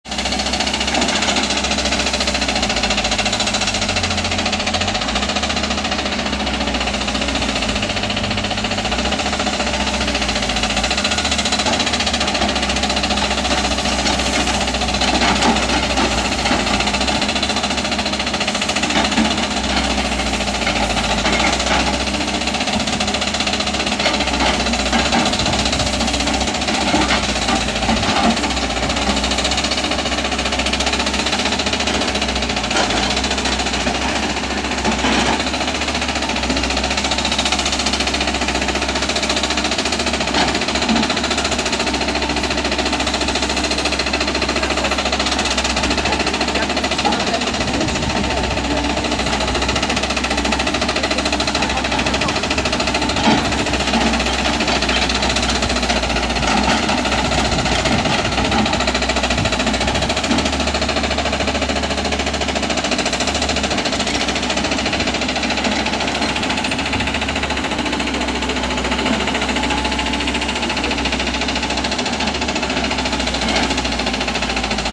Na Kadzielni trwa budowa 21.06.2018
Na Kadzielni trwa budowa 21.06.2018. Zdjęcia i więcej informacji na stronie